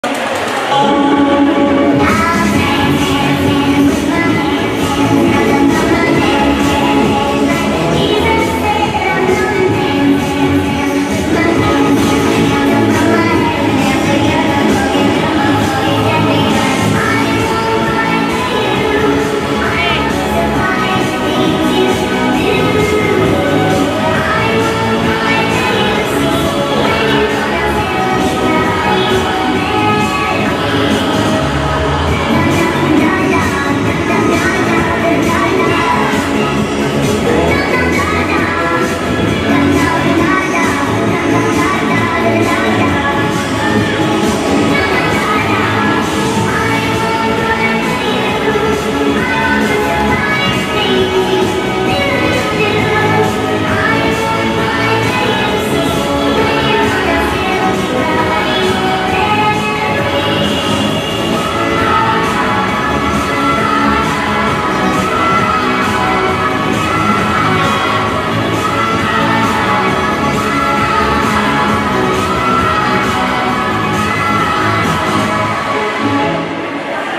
Pom Pom Cadet Solo- Lucia Blagovčanin- Plesni klub Fiume, Croatia
WAMT World Championship 2024.
Pom Pom Cadet Solo
Lucia Blagovčanin
Plesni klub Fiume
Croatia